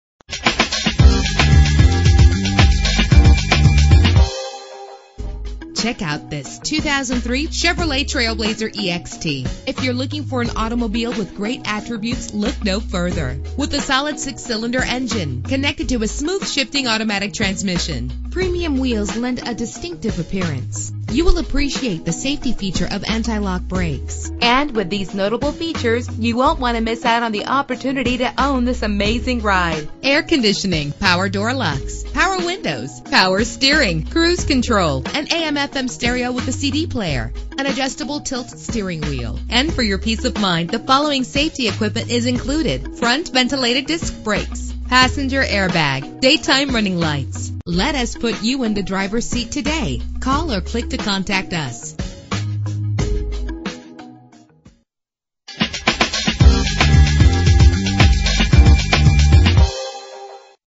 2003 Chevrolet TrailBlazer EXT - Warminster PA
Year: 2003
Make: Chevrolet
Model: TrailBlazer EXT
Trim: LS
Engine: 4.2 liter inline 6 cylinder 24 valve
Transmission: 4-Speed Automatic
Color: Carbon Gray Mist
Mileage: 75569
Address:
869 West Street Rd